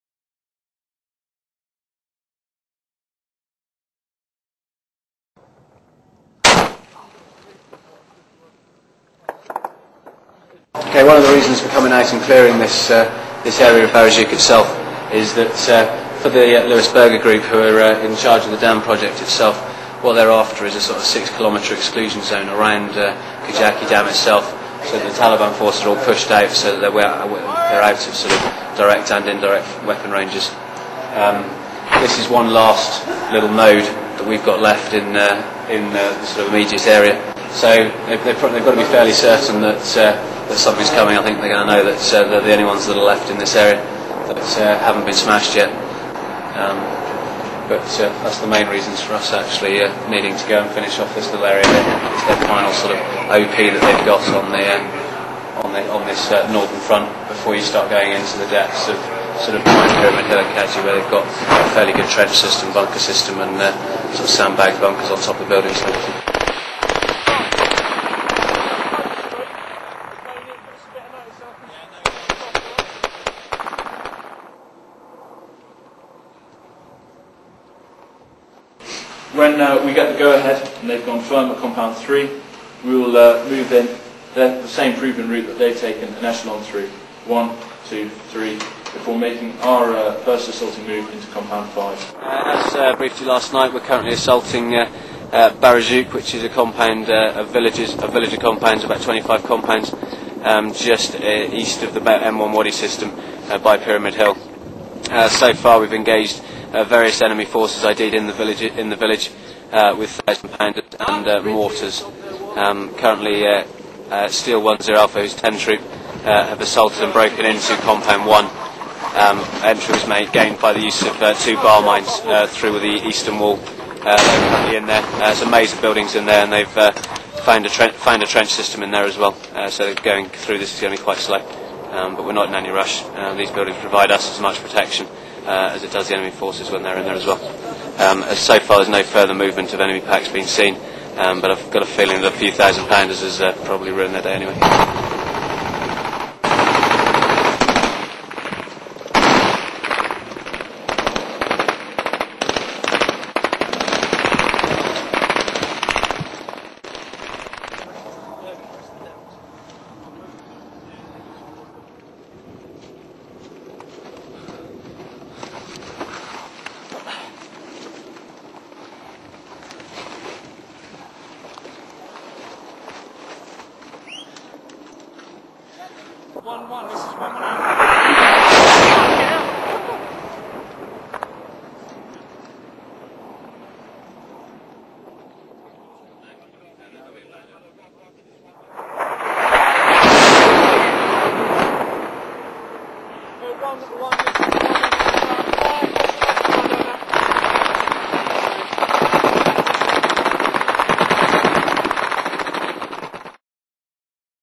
Operation Volcano
Royal Marines Assault Taliban positions near the Kajaki Hydro-Electric Damn.  Video courtesy of the Ministry of Defense.